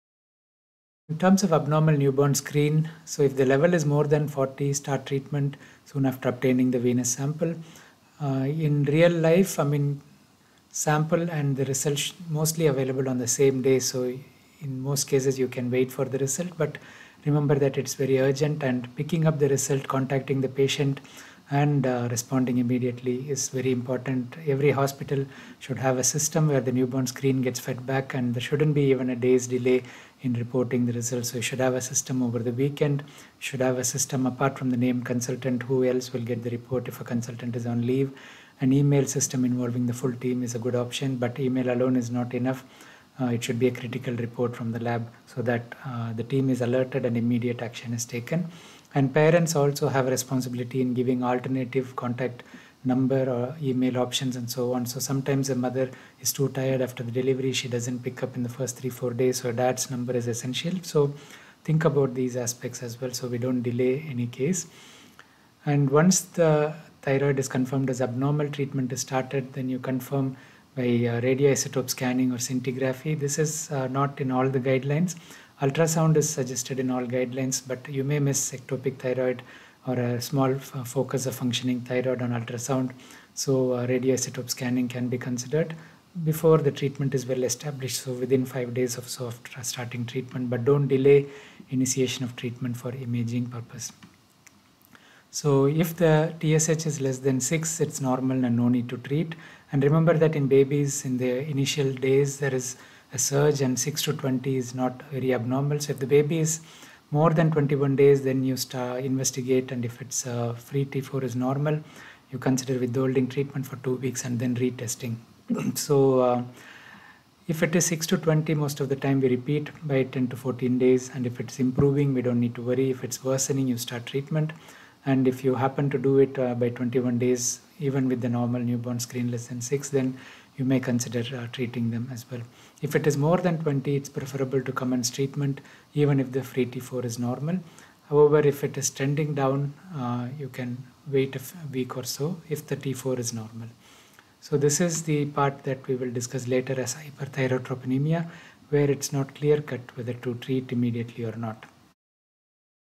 Abnormal newborn screening l Congenital hypothyroidism
In this video, I discuss the approach to abnormal newborn screening for congenital hypothyroidism.